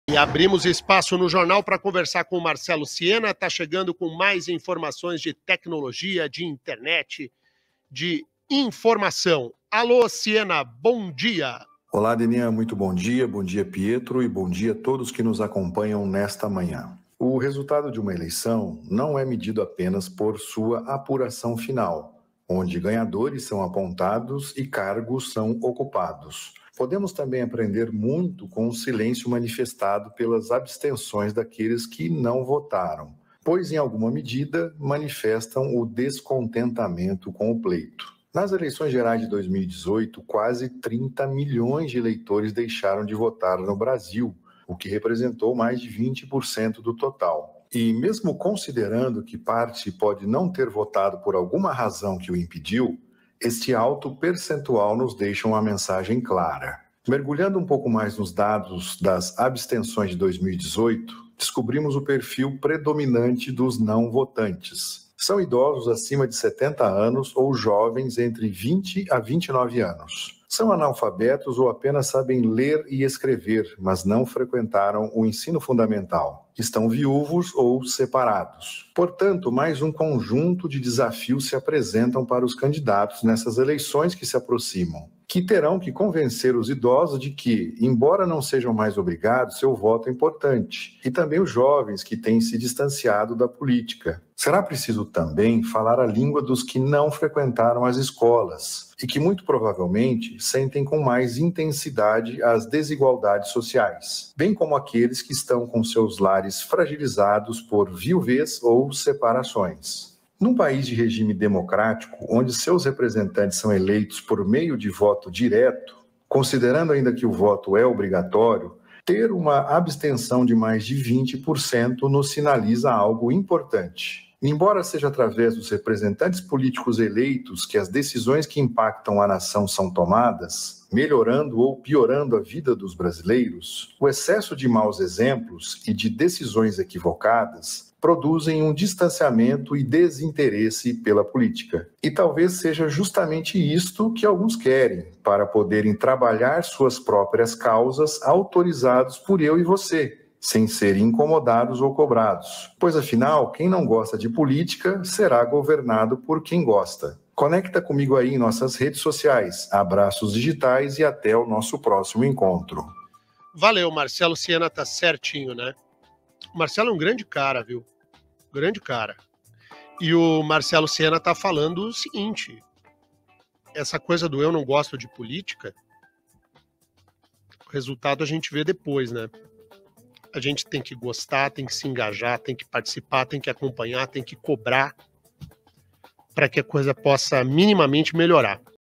Marcelo Siena fala sobre a importância do voto dos jovens e idosos nas eleições de 2022
Dra. Cristiane Paulin - Harmonização Orofacial